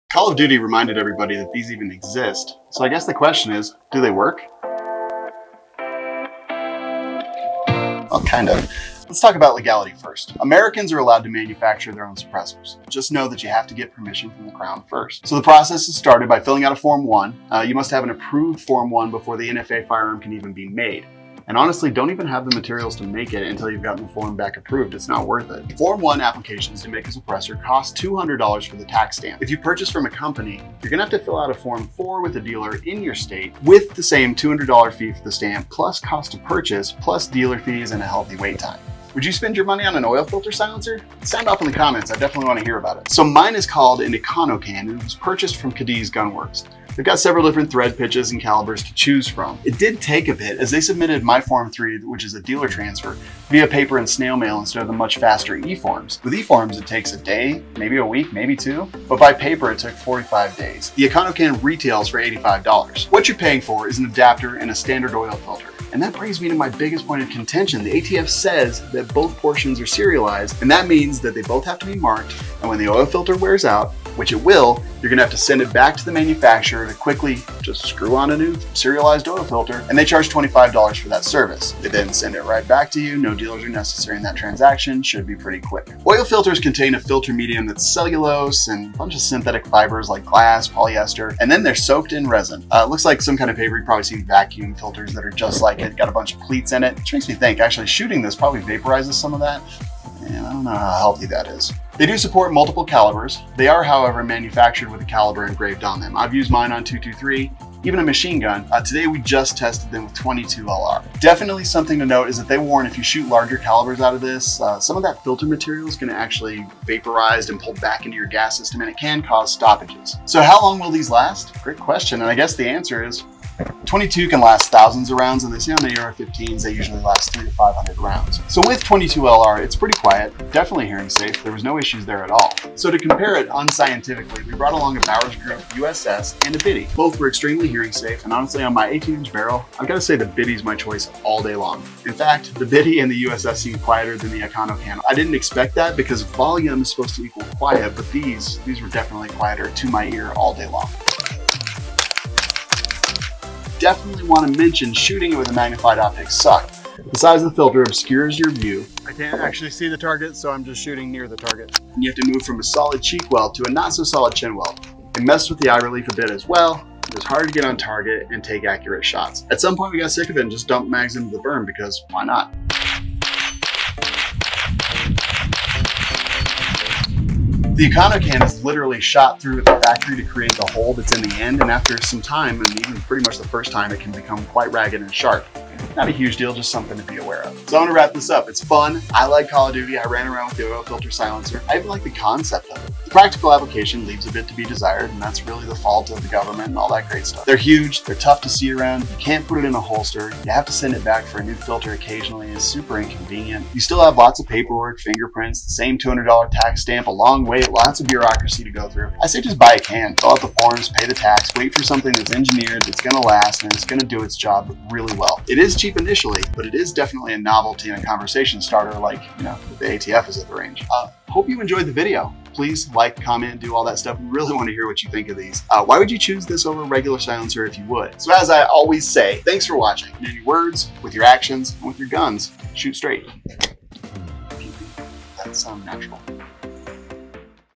Do Oil Filter Silencers work?
Is an oil filter silencer legal? Watch and see! This is the EconoCan.